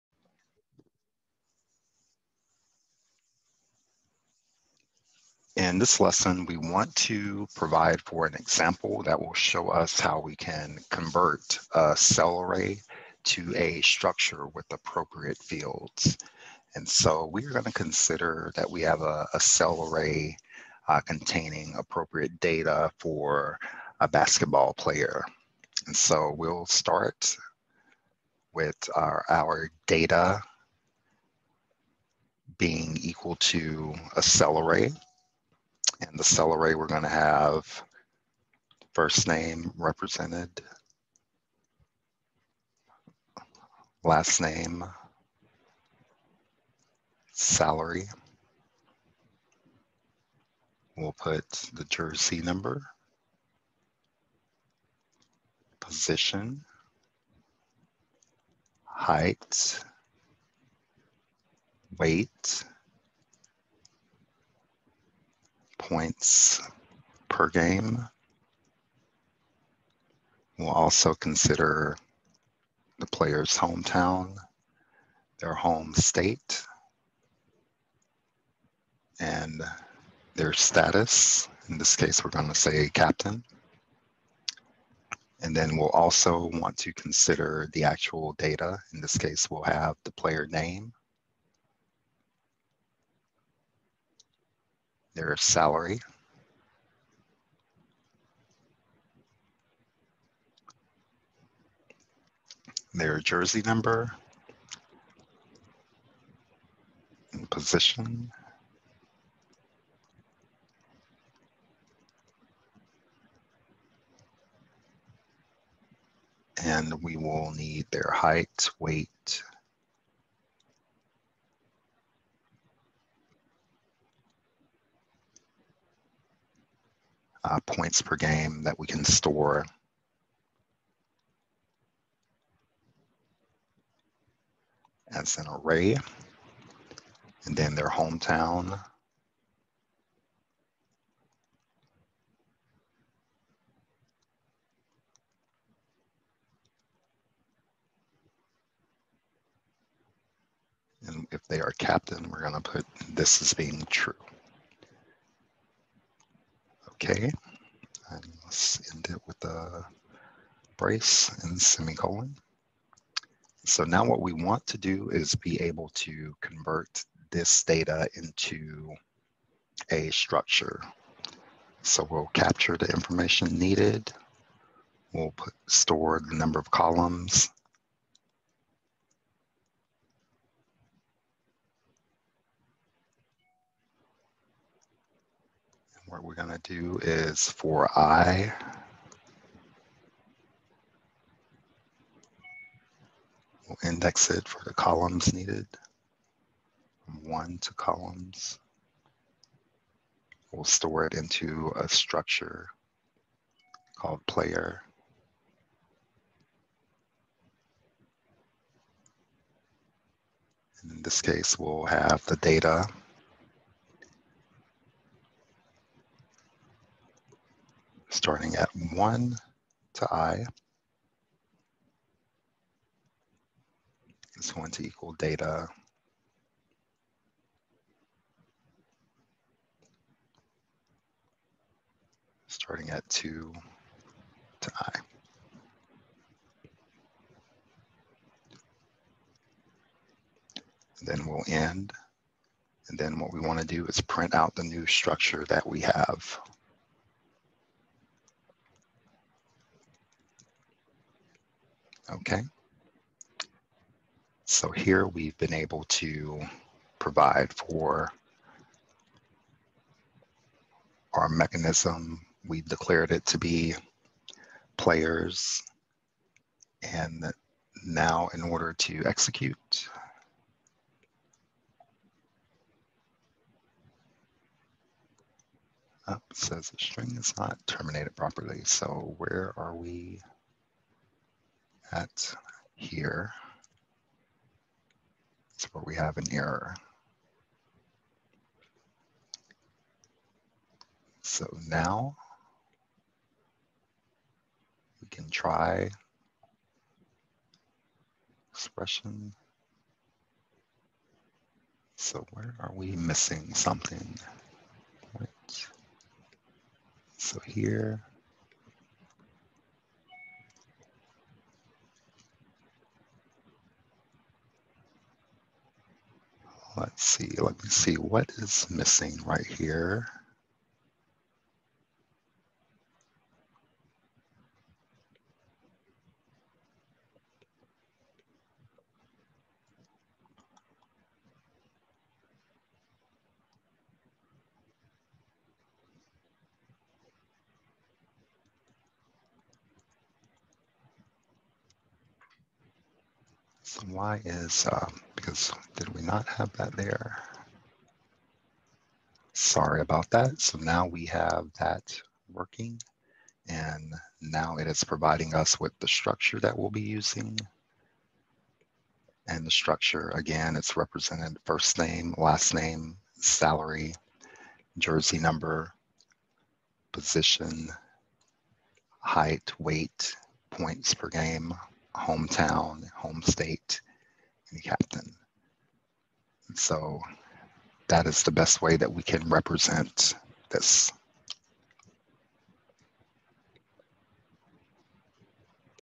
Matlab: Convert a Cell Array to a Structure
In this video, we demonstrate how to convert a cell array into a data structure.